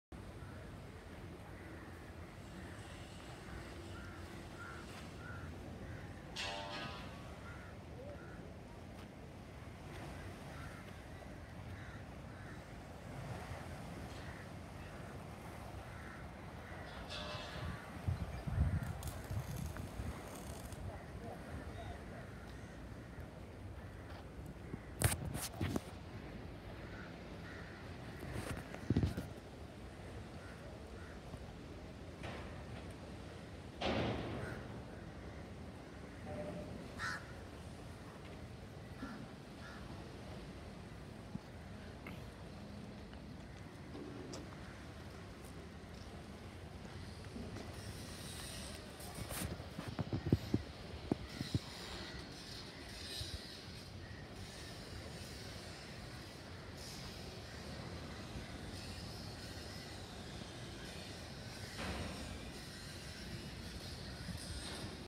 Walkers Colombo Shipyard
Docking of a trawler today